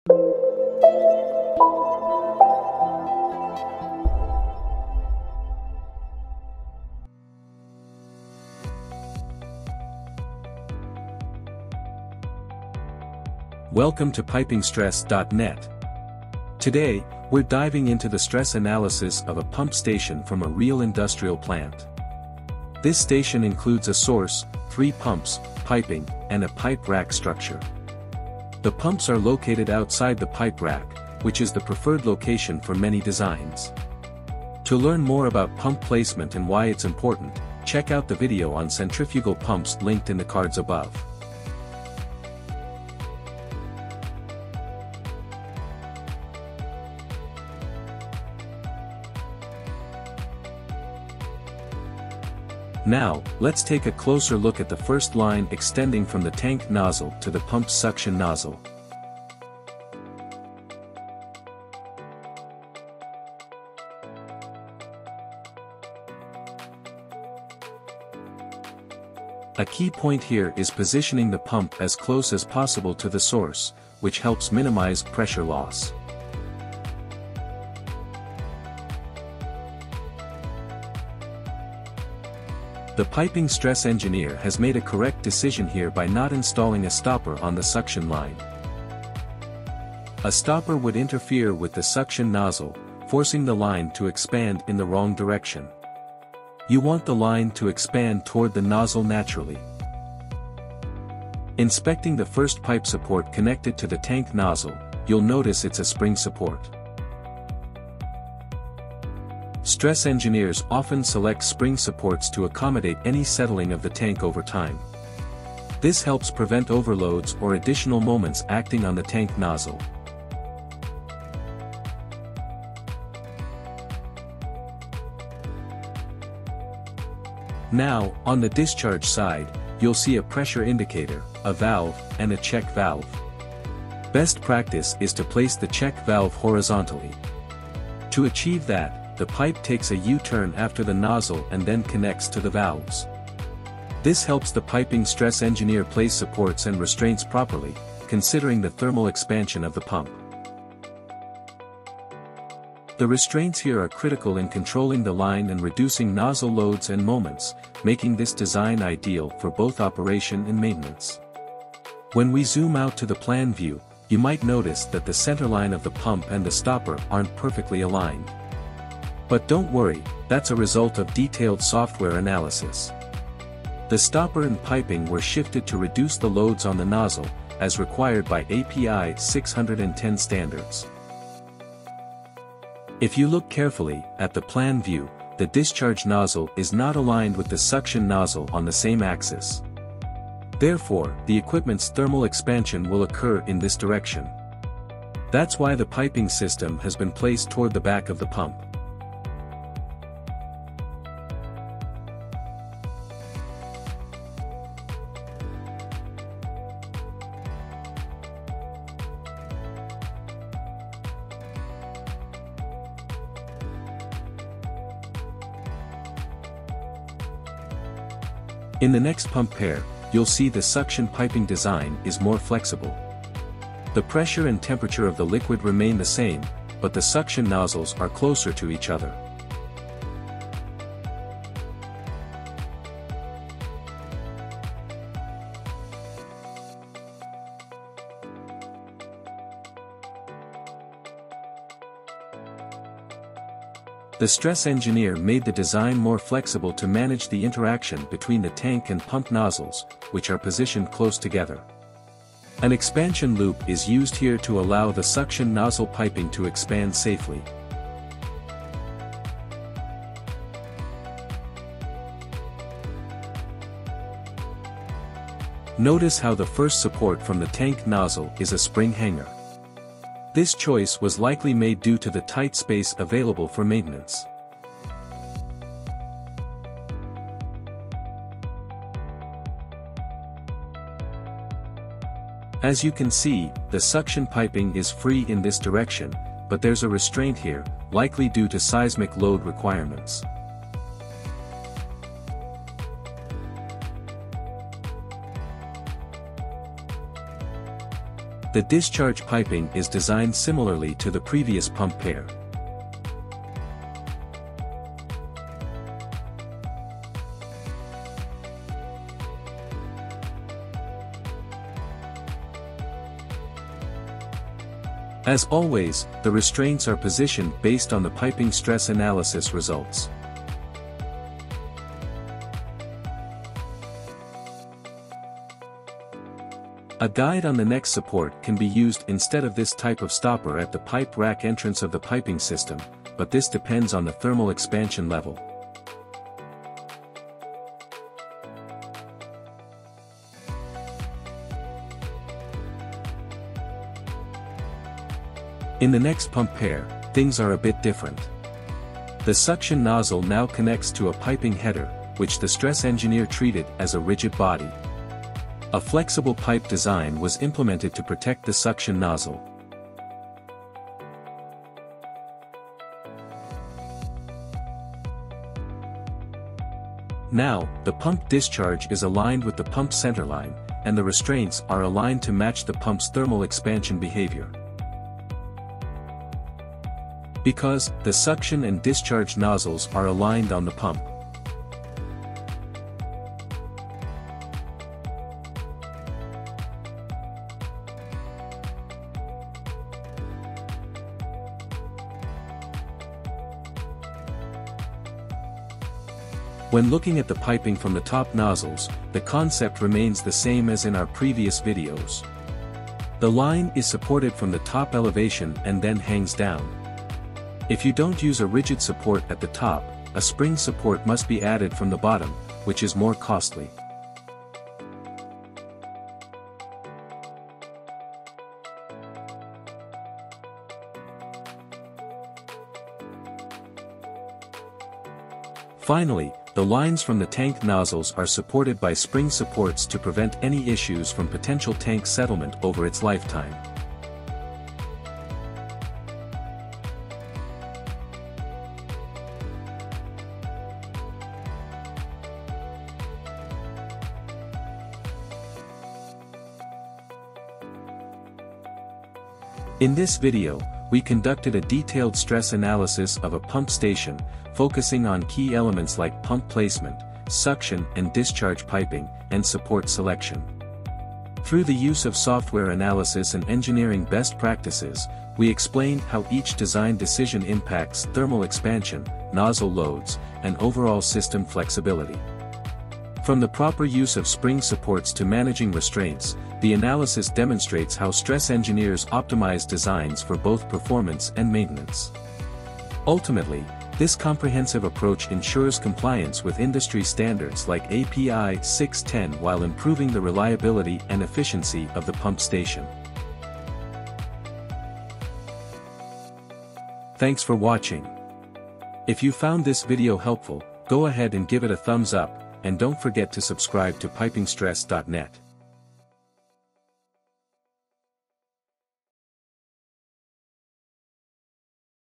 Pump Station Piping Design and Stress Analysis
This video includes an actual pump station consisting of pumps, a tank, piping, and a pipe rack. It shows how the piping system is properly supported as per code requirements.

You can find more information about piping stress engineering, piping design, and piping construction from the link below;